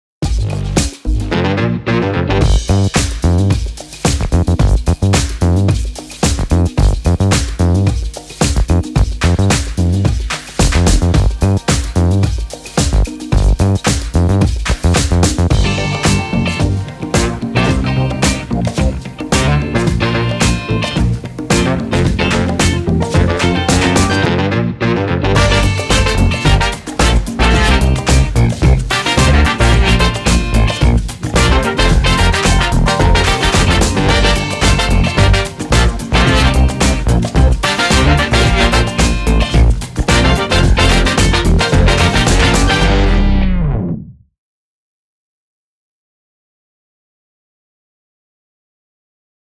Loupe support for Sony FX6 - Flop Stop monitor fix!
Utilising 32 neodymium magnets, the Flop Stop reinforces the FX6 monitor bracket to prevent it from drooping sideways and crucially enables the use of an FS7 or FX9 loupe.